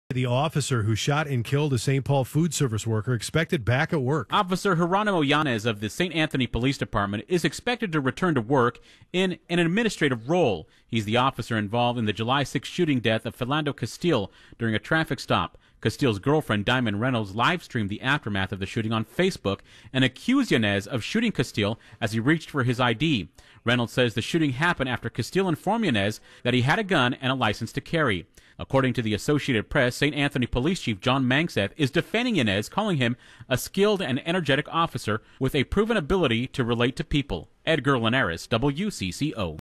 Officer In Castile Shooting Back At Work Wednesday (Audio)
St. Anthony’s police chief defended his officer who shot and killed a black man during a traffic stop as level-headed and skilled in an interview with The Associated Press Wednesday. More than a month later, St. Anthony Police Officer Jeronimo Yanez was expected to return to work for the first time Wednesday, Chief Jon Mangseth said.